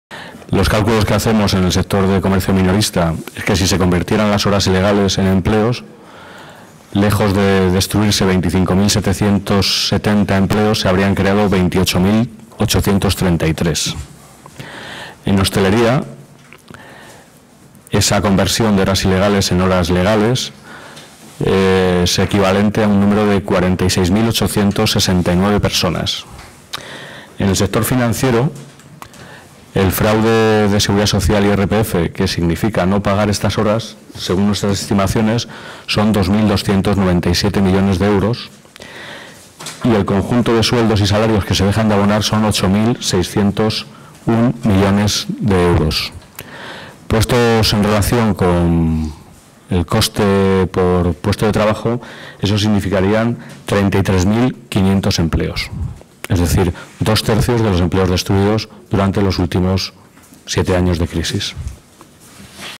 Horas ilegales algunos datos José María Martínez Servicios CCOO
Con las horas ilegales, en el sector de comercio minorista so podrían haber creado 28833 empleos. En hosteleria sería equivalente a 46869. en el sector financiero el fraude a la Seguridad Social y el IRPF 2297 millones de Euros y 8601 milloes de euros en salarios que se dejan de abonar. Eso implicaria 33.500 empleos, dos tercios de los empleos destruidos durante los ultimo siete años de crisis